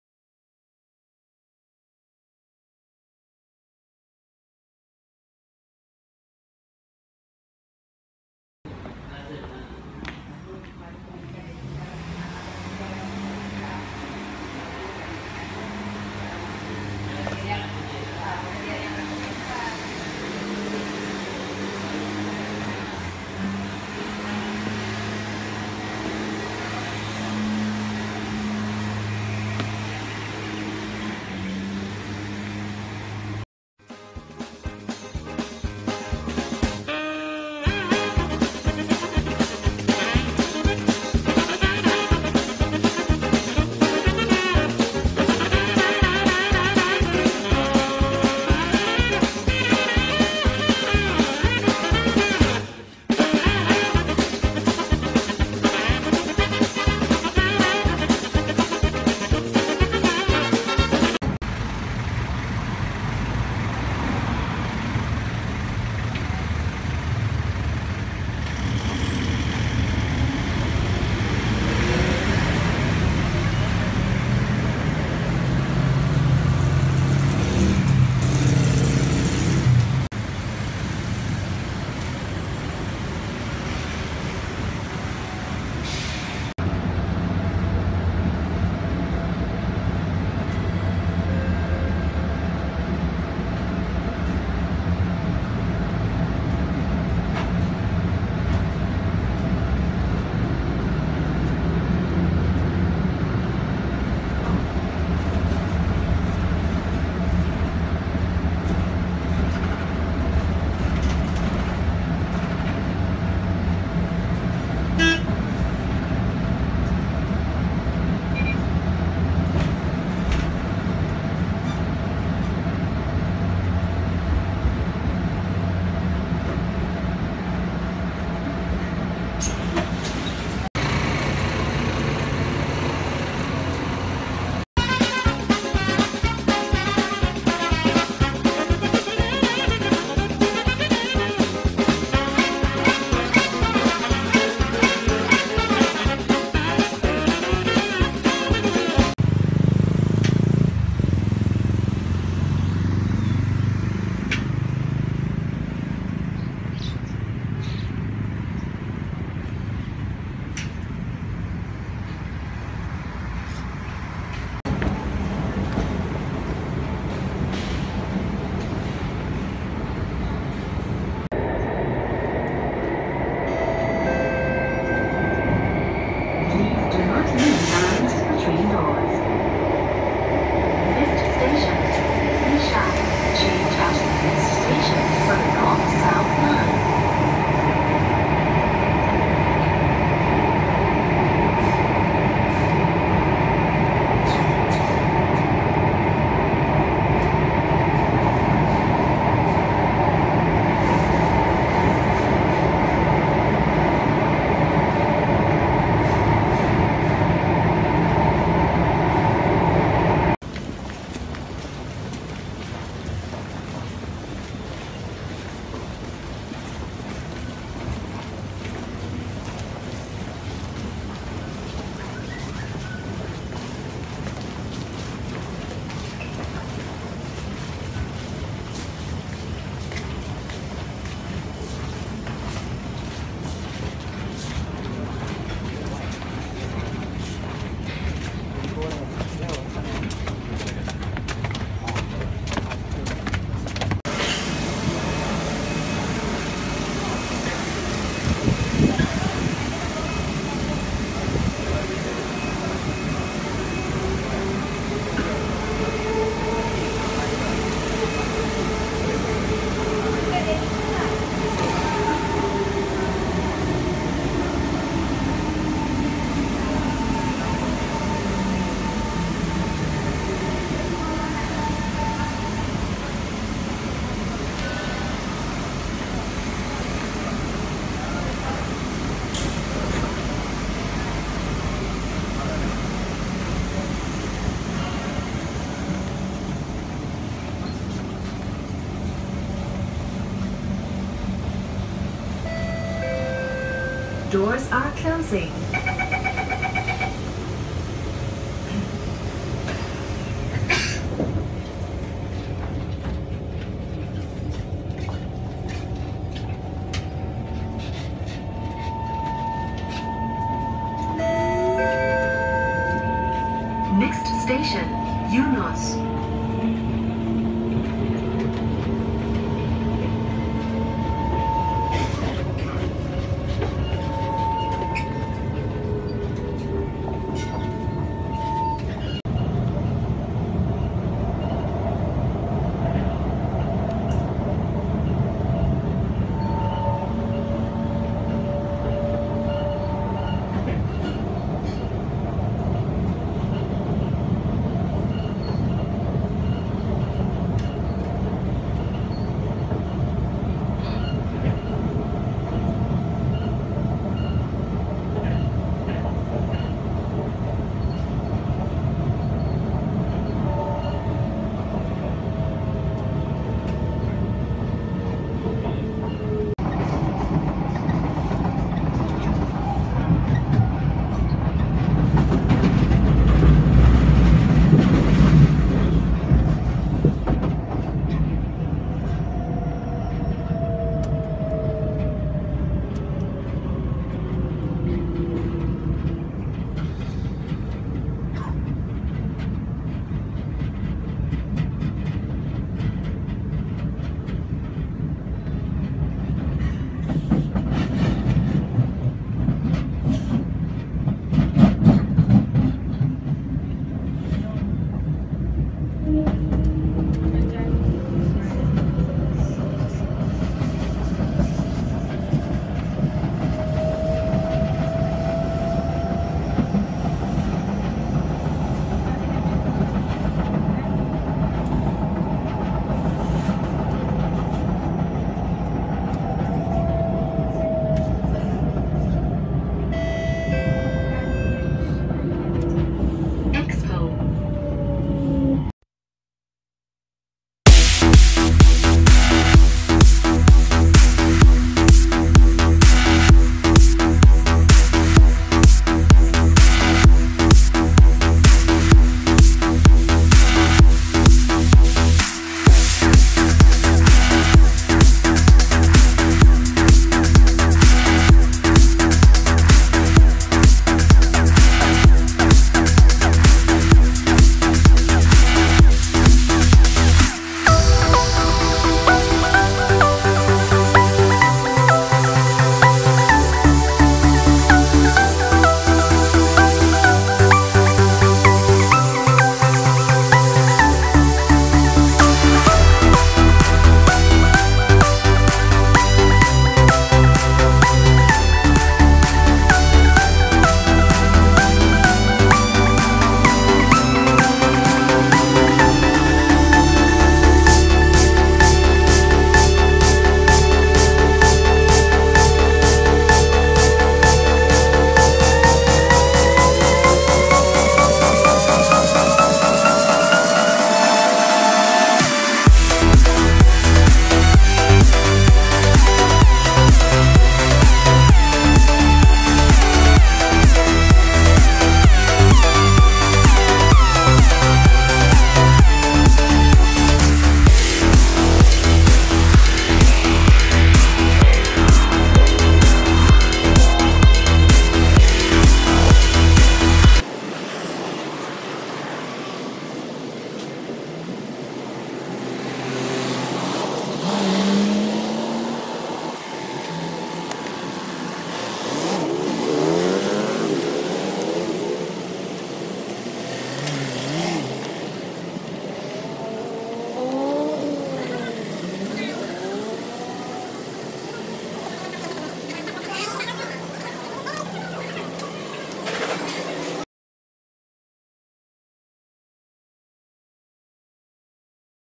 Documentary of my daily commute to work (Around 1.5 hour)
A combination of bus and train trip to work from Monday to Friday.
A typical life of a salary-man.